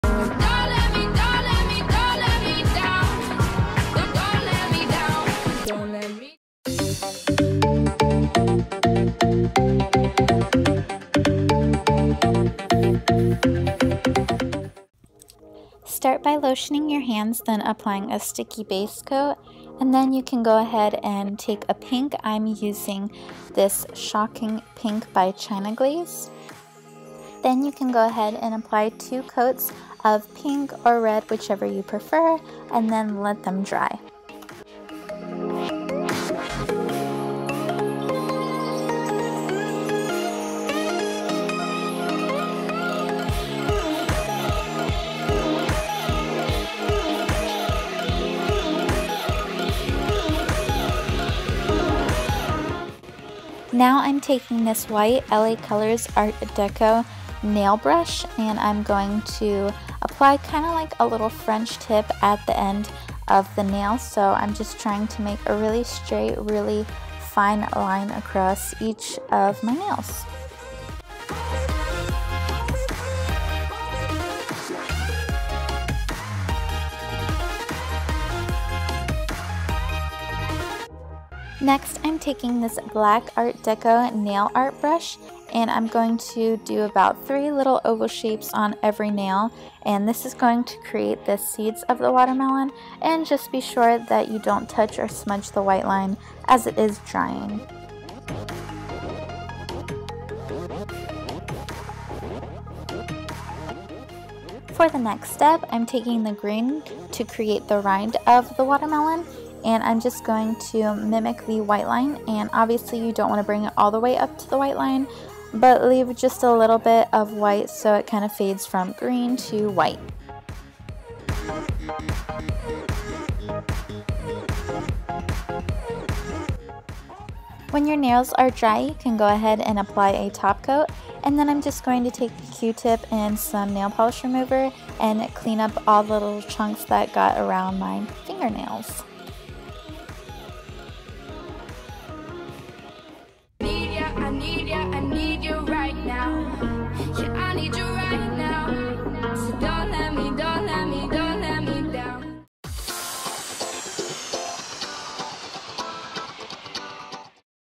Easy Summer Nails | Watermelon Nails
Check out this tutorial on these easy summer watermelon nails!